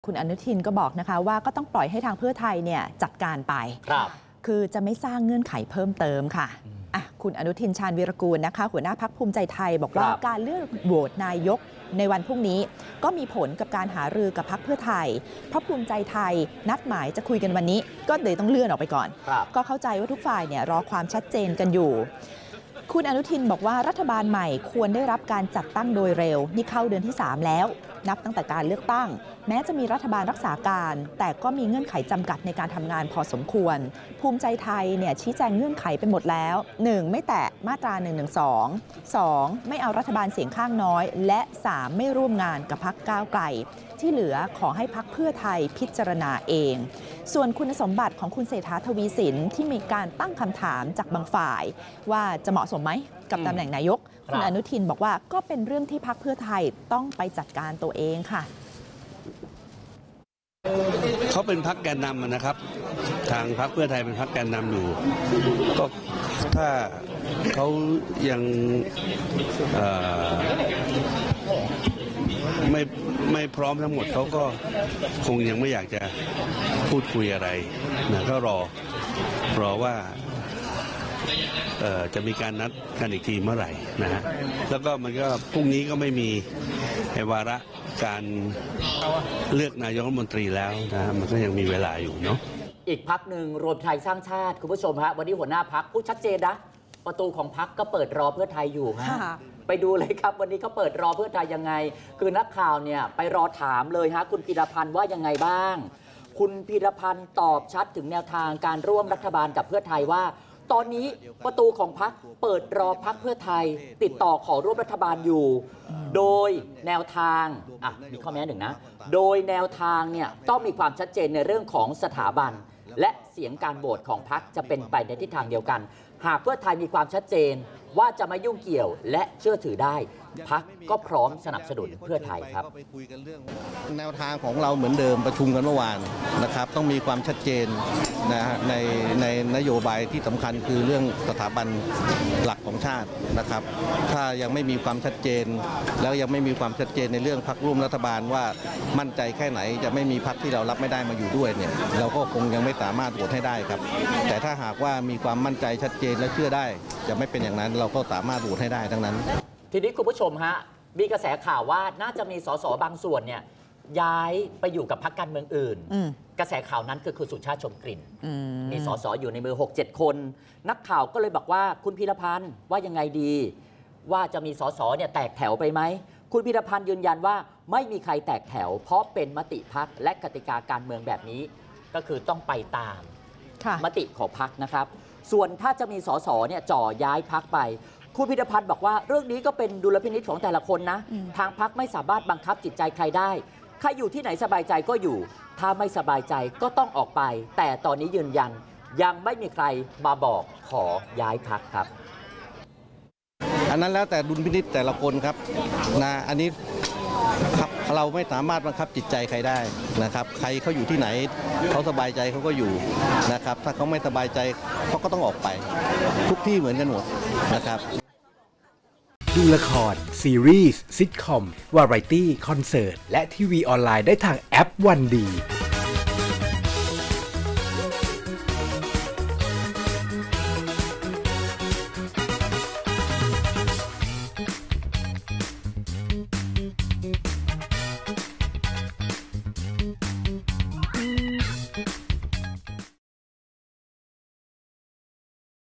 "พีระพันธุ์" เปิดประตูรอ "เพื่อไทย" ชี้ช่องร่วมรัฐบาล | ข่าวเย็นช่องวัน | สำนักข่าววันนิวส์
"พีระพันธุ์" หัวหน้าพรรครวมไทยสร้างชาติ ลั่นไม่ปิดประตูร่วมงาน "เพื่อไทย" ย้ำหากแนวทางตรงกันพร้อมโหวตให้ ไม่ง้อกระแส สส. จ่อย้ายพรรค บอกสบายใจที่ไหนก็อยู่